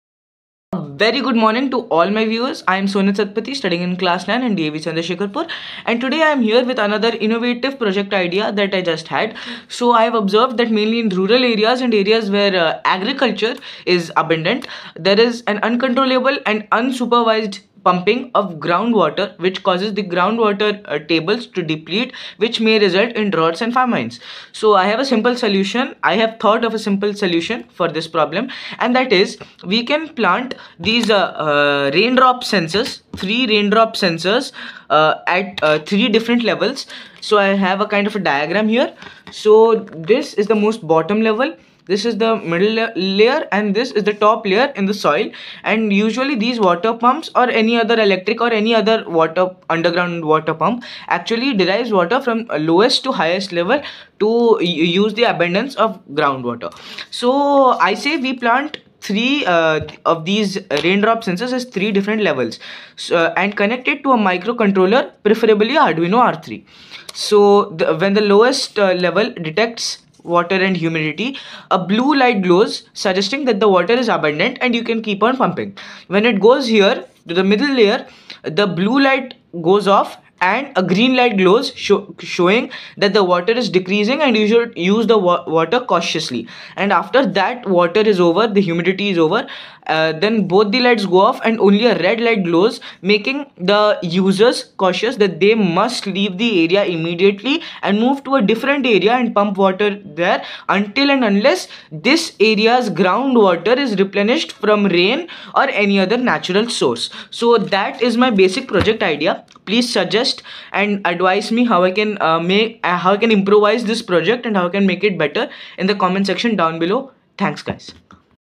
Jala Rakshak System
Problem Statement:
Groundwater depletion is a major global issue with severe consequences, including:
● Water Scarcity: Reduced access to clean, fresh water for billions of people.
● Agricultural Impact: Lower crop yields, leading to food insecurity and increased
risk of famine.
● Environmental Consequences: Land subsidence, reduced stream flows, and
ecosystem degradation.
● Economic Impact: Increased costs for water extraction, affecting livelihoods.
● Health Risks: Increased consumption of contaminated water due to scarcity.
● Climate Change: Groundwater depletion exacerbates the impacts of droughts.
Groundwater is a vital resource, providing about 30% of the world's fresh water and
supporting agriculture, domestic use, and industrial activities.

Solution Statement:
The Jala Rakshak System is a proposed solution to monitor and prevent groundwater
depletion.

System Overview:
● Installation: A thin hole is dug near water pumps or irrigation systems.
● Sensors: Three humidity sensors are installed at different levels within the hole.
Working Mechanism:
● Blue Light: All three sensors detect adequate moisture, indicating stable
groundwater levels.
● Green Light: Bottom sensor stops detecting moisture, signaling the beginning of
depletion.
● Red Light: Middle sensor stops detecting moisture, indicating critical depletion.
Triggers a blinking red light, alerting users to stop extraction and move to a
different location.

Benefits:
● Prevents groundwater depletion through real-time monitoring.
● Ensures sustainable water management and reduces water scarcity risks.
● Lowers costs associated with drilling deeper wells and increases water use efficiency.
● Creates jobs for system management and control.
● Protects the environment by preventing land subsidence and safeguarding
ecosystems.

Future Scopes:
● Integration with IoT for remote monitoring and automated alerts.
● Data analytics to inform advanced water management and policy decisions.
● Community education on sustainable groundwater use and conservation practices.
Improvement in Day-To-Day Life:
○ Agricultural productivity: Provides reliable irrigation, improving crop
yields and food security.
○ Infrastructure protection: Reduces the risk of land subsidence,
safeguarding homes and infrastructure.
○ Economic stability: Creates job opportunities and fosters community
involvement.
○ Environmental conservation: Promotes water conservation and
efficient usage, contributing to a sustainable lifestyle.
Conclusion
The Jala Rakshak System is a practical and innovative solution for monitoring and
preventing groundwater depletion. By implementing this system and promoting household
contributions, we can ensure the long-term availability of groundwater resources for both
rural and urban communities.